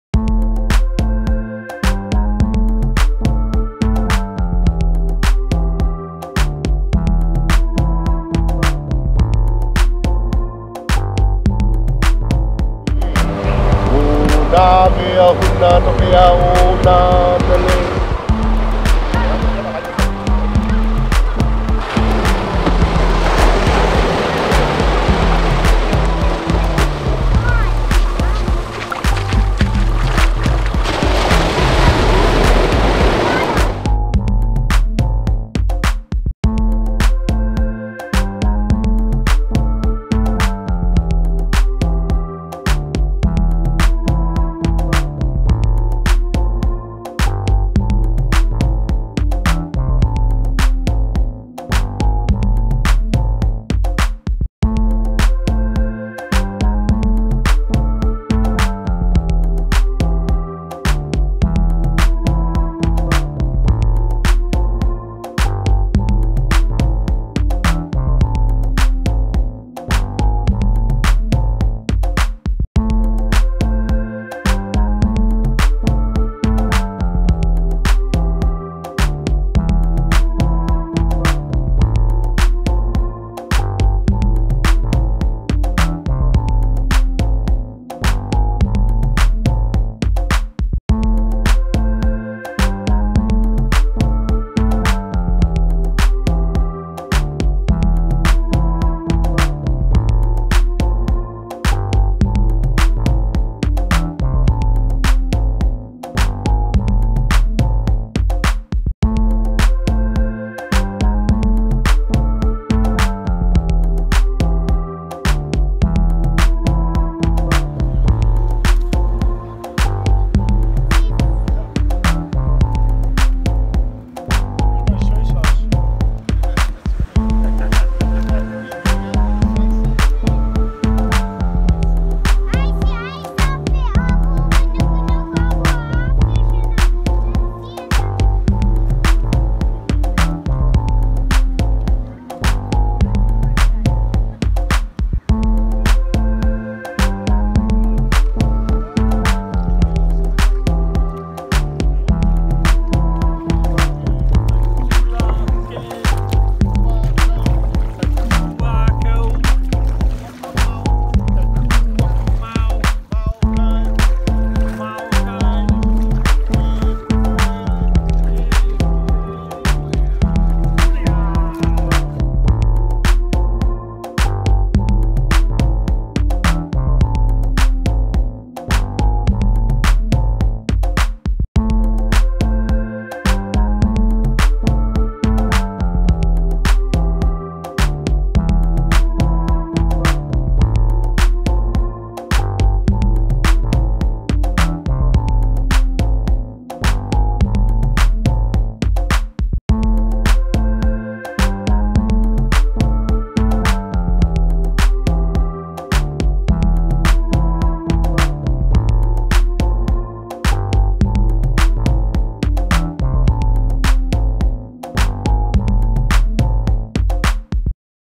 The2Woodies - Canoeing in Maui
Join Carter and Tucker and their friend Emily as they head out on a canoe with Kevin from Hawaiian Ocean Experience.